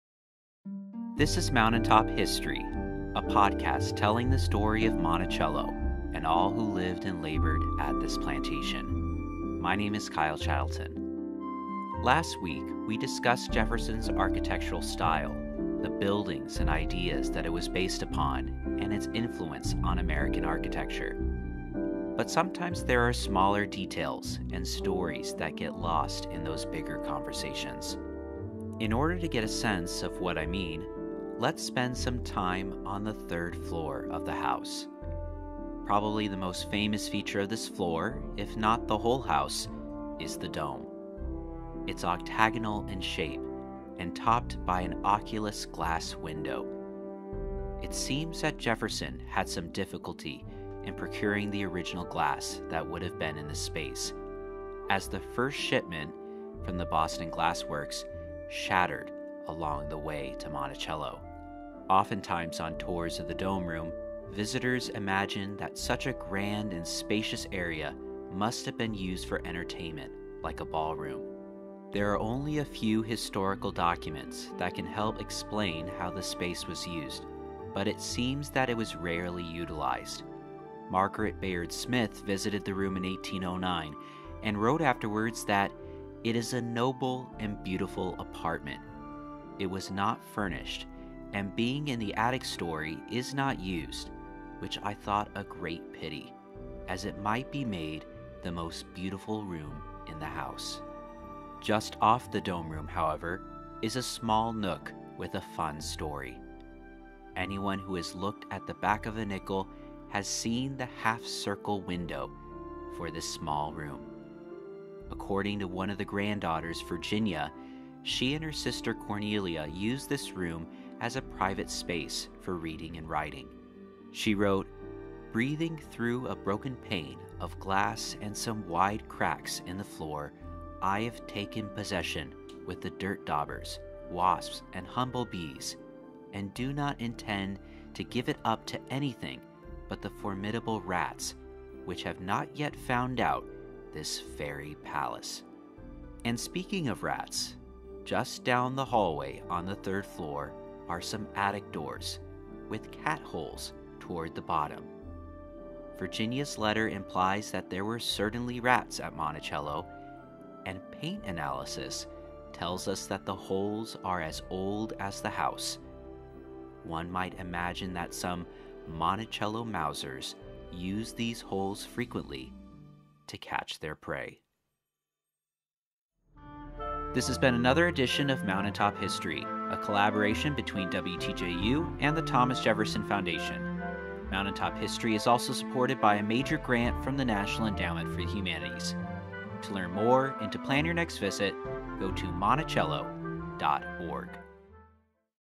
Dome Rooms, Cat Holes, and Fairy Palaces
In a recent episode, we focused on the more influential aspects of Jefferson’s architecture. But in a conversation like that we can miss out on other details. Monticello guide Kyle Chattleton shares with us how the nooks and crannies of the third floor of Jefferson’s home can point to smaller, yet still memorable stories.